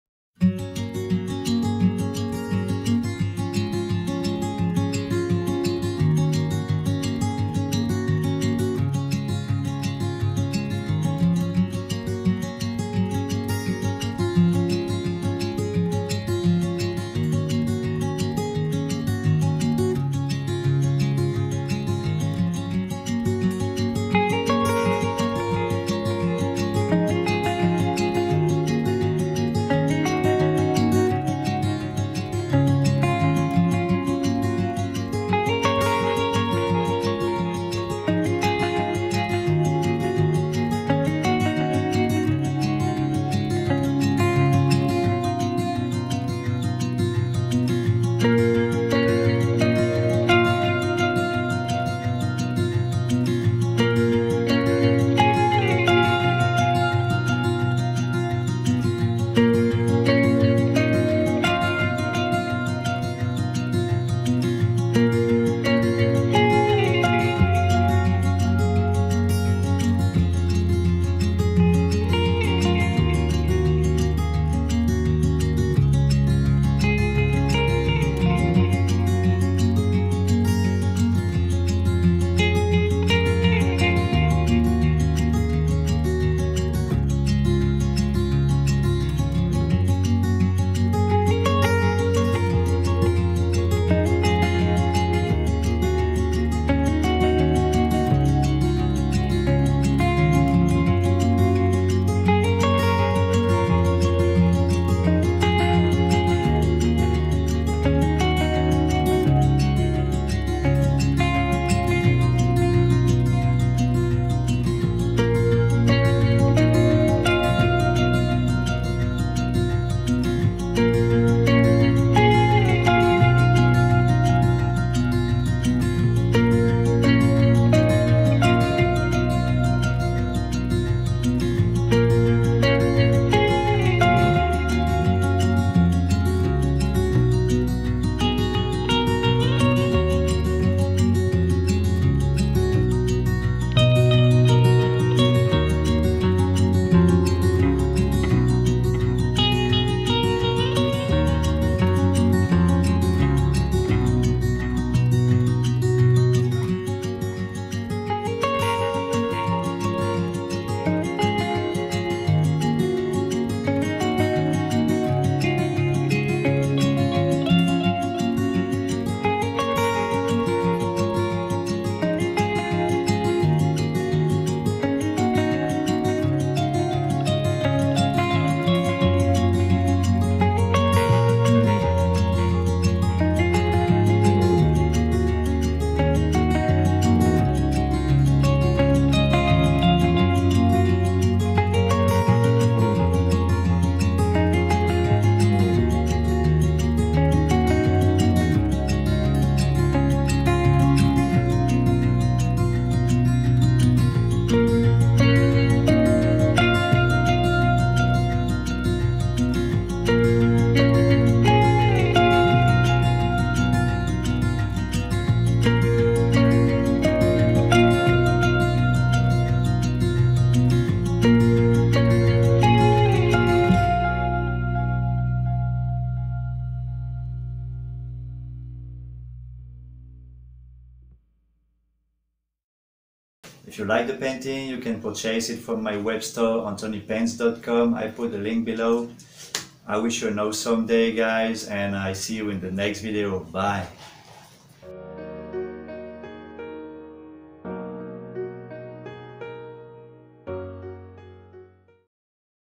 Reading under the stars . Glow in the Dark . Spray Paint Art . by Antonipaints Art
Hey What's up guys ! I made this painting with glow in the dark paint .
Enjoy the video and leave me your comments below .

Music Without Copyright 100% Free

Music & Arrangement : Gjorgjievski Marjan
Mix & Mastering : Gjorgjievski Marjan
Assistant : S.Teov
Category
Music